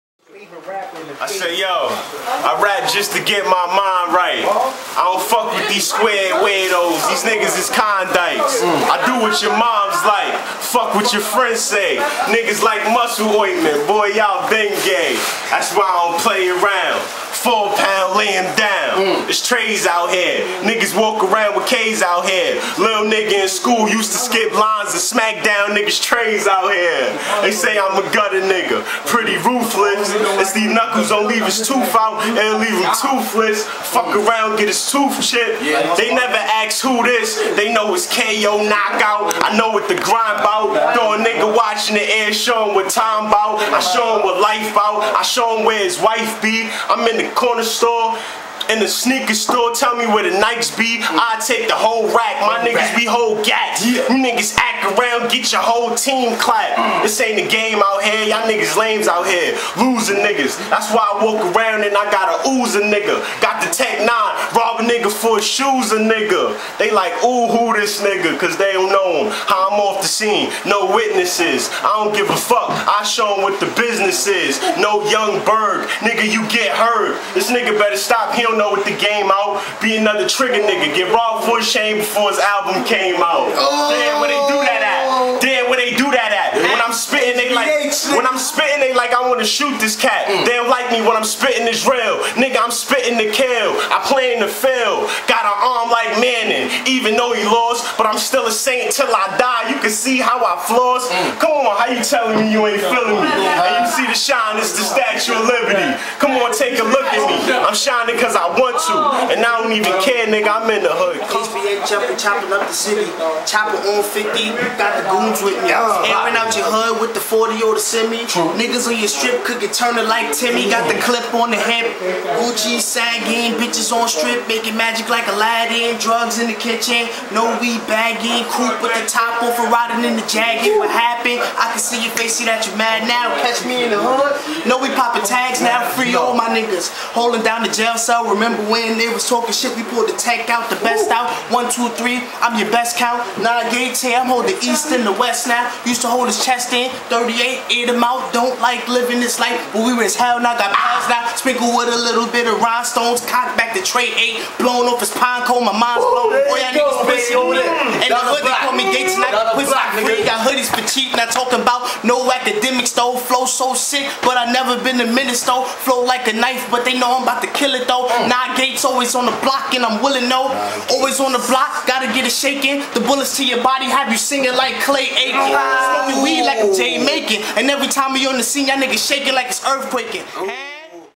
2010 Flow Pt 2
Da Tag Team Champs Are Bacc At It Again. Bringin You Dat Crazi Flow Plus They Got Stupid Bars. KO And Niyy Gates.Who Wannna BATTLE